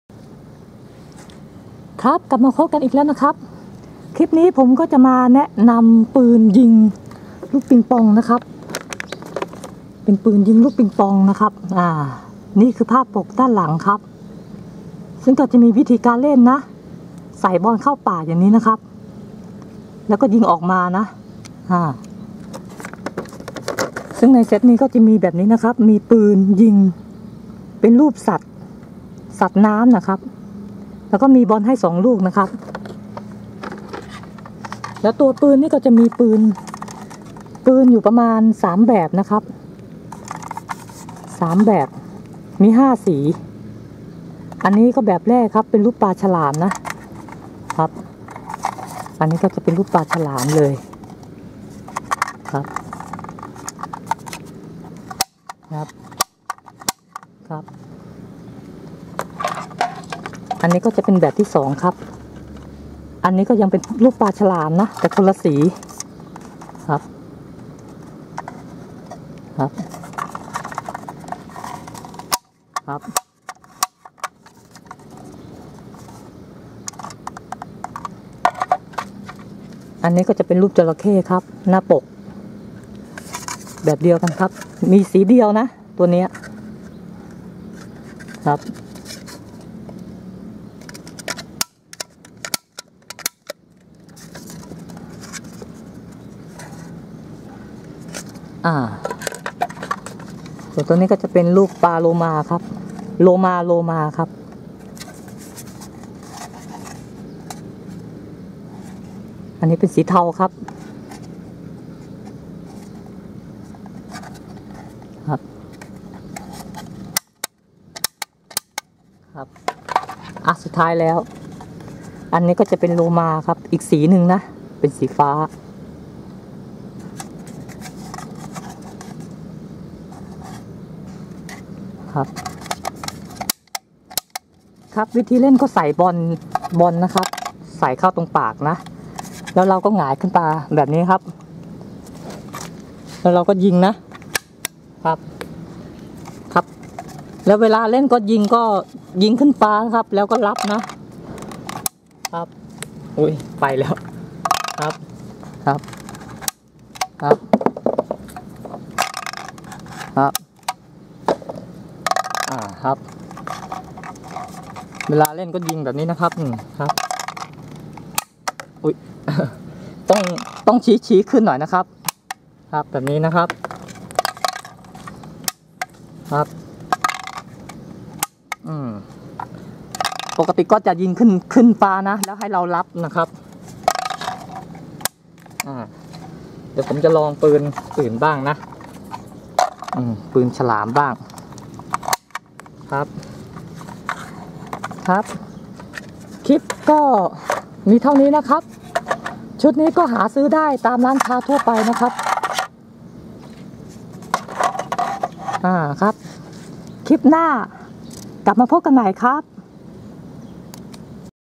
ของเล่นปืนยิงปิงปอง Crazy Shark
ยิงขึ้นฟ้าแล้วรับบอลที่ตกลงมา